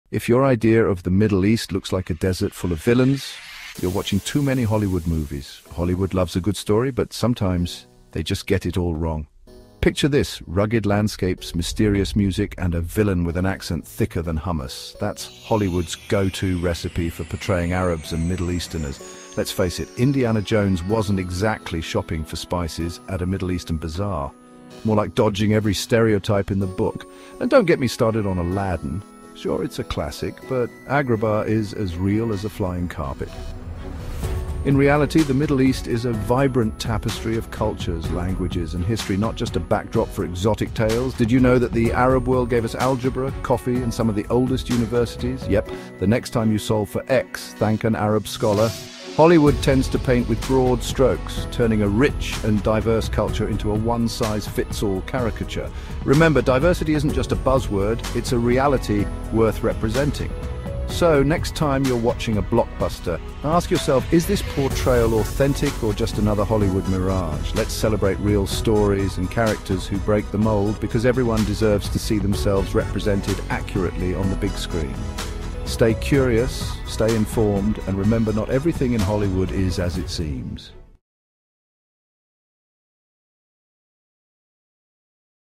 Killing the Hollywood Stereotypes About the Middle East
If your image of the Middle East comes from Hollywood, you might be picturing deserts, villains with thick accents, and mysterious music. But Hollywood often gets it wrong. This video breaks down the clichés seen in movies like Indiana Jones and Aladdin, where a rich and diverse region is reduced to tired stereotypes. The reality? The Middle East is a vibrant tapestry of cultures, languages, and groundbreaking achievements—from algebra to some of the oldest universities in the world. It’s time to look beyond the Hollywood mirage and celebrate real stories that reflect the true diversity of the region. Next time you're watching a blockbuster, ask yourself: is this portrayal accurate, or just another exaggerated caricature?🌟📽️